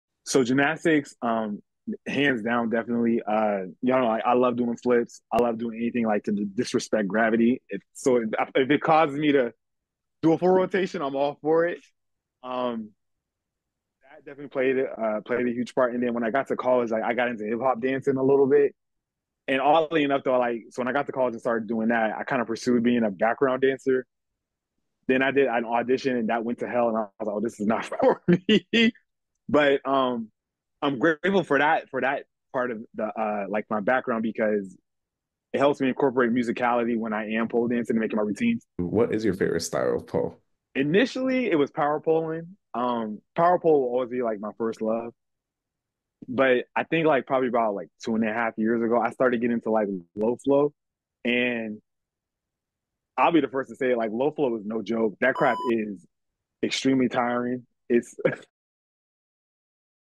André Vertical Jones Loves Power Pole Dance & Low Flow Styles
We asked @vertical.jones what his favorite pole dance style is, and in this clip he reveals it is power pole & low flow 🤩

André is a pole dance champion and coaches at @liegebarreandpole

Learn more about this incredible pole dancer in our new podcast interview premiering tomorrow!